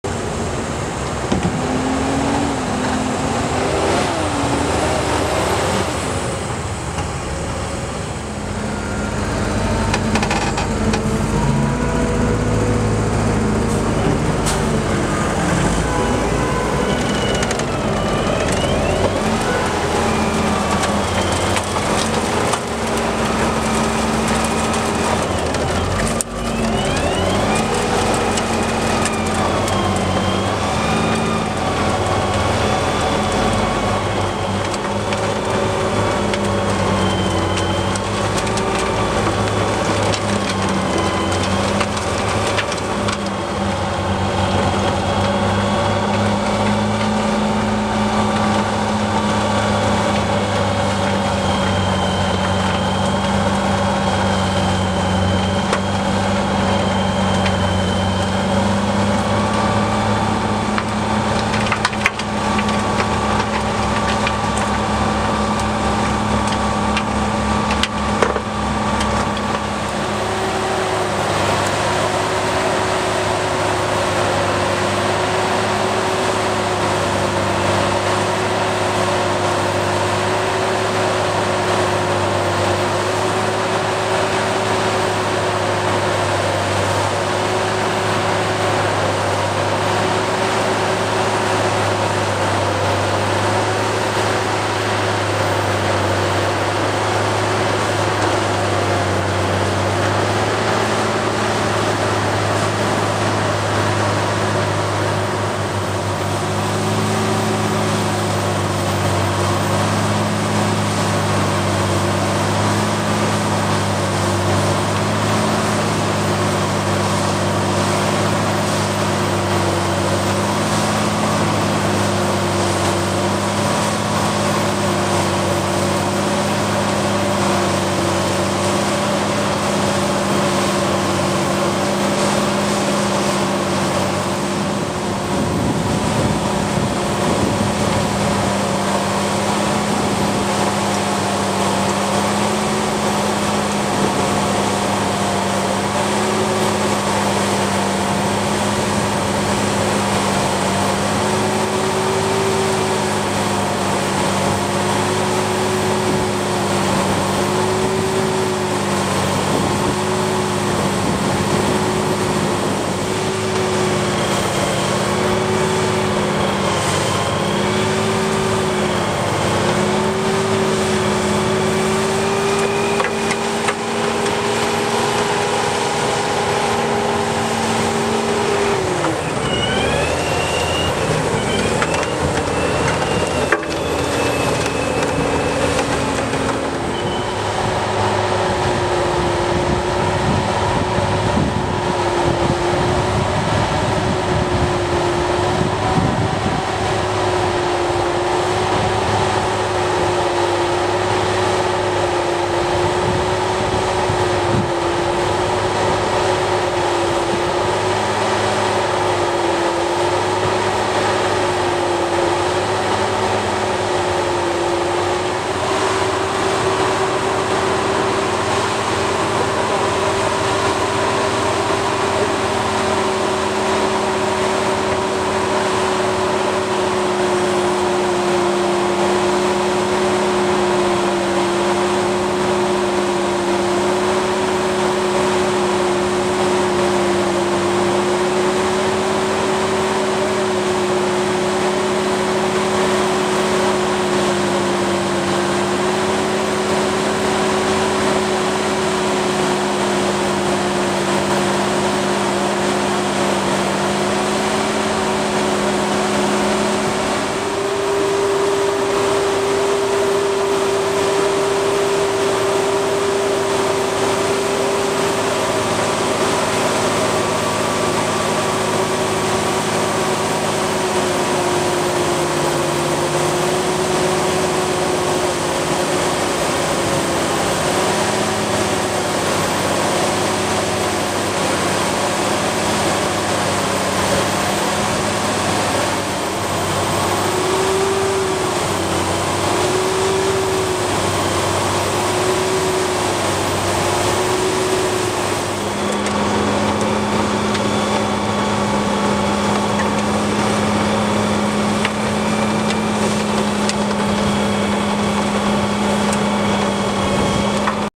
FENDT 936 Vario mit Anbaufräse Wirtgen WS 250, Weiler zum Stein, Germany, 14.02.2017.
FENDT 936 Vario + Wirtgen WS 250 tractor-towed stabilizer / Anbaufräse, Weiler zum Stein, Germany, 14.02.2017.